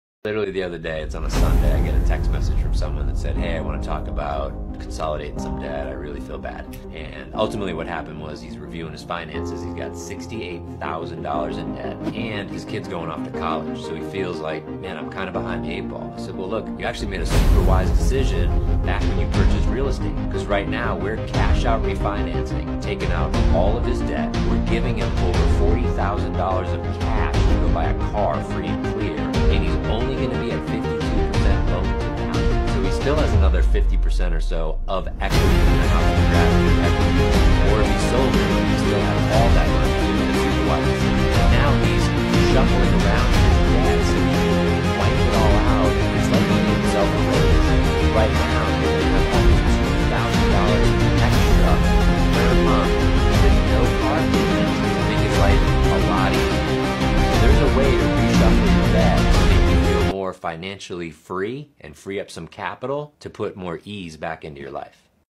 Financial freedom is within reach! ⠀⠀⠀⠀⠀⠀⠀⠀⠀ Recently, a client reached out feeling overwhelmed by $
Financial freedom is within reach! ⠀⠀⠀⠀⠀⠀⠀⠀⠀ Recently, a client reached out feeling overwhelmed by $68,000 in debt as their child prepared for college. 💸 ⠀⠀⠀⠀⠀⠀⠀⠀⠀ Through a strategic cash-out refinance, we cleared their debt and even provided over $40,000 cash for a car, all while maintaining a comfortable loan-to-value ratio. ⠀⠀⠀⠀⠀⠀⠀⠀⠀ Let's explore how you can reshape your debt for financial ease. Reach out to learn more! 📞💰 ⠀⠀⠀⠀⠀⠀⠀⠀⠀ ⠀⠀⠀⠀⠀⠀⠀⠀⠀ ⠀⠀⠀⠀⠀⠀⠀⠀⠀ ⠀⠀⠀⠀⠀⠀⠀⠀⠀ ⠀⠀⠀⠀⠀⠀⠀⠀⠀